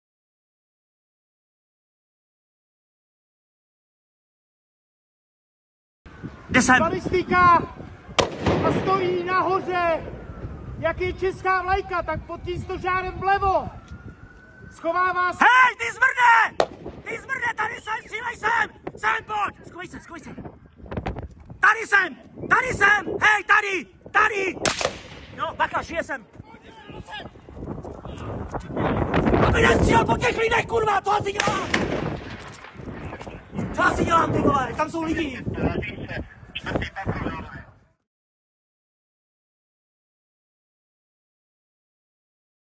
Brave man shouts 'shoot here you f***er' in bid to distract Prague shooter from targeting students
Footage of the Prague shooter has emerged of him standing on the balcony of the Charles University aiming his powerful ZEV-30 rifle at people below, picking his victims off one by one.

David Kozak, wearing all black, can be seen walking calmly along the balcony and taking aim at the terrified students fleeing the university and tourists who had been gathered in the popular square below.

On the ground, a heroic man can be heard screaming 'shoot me you f***er' in a desperate and valiant bid to distract the shooter and save the lives of others, reports newspaper Blesk.

But the 24-year-old ignores his pleas and continues to fire in quick succession at his victims below as the man frantically tells police where exactly Kozak is located on the balcony through a walkie-talkie, urging them to 'take a shot' at the shooter.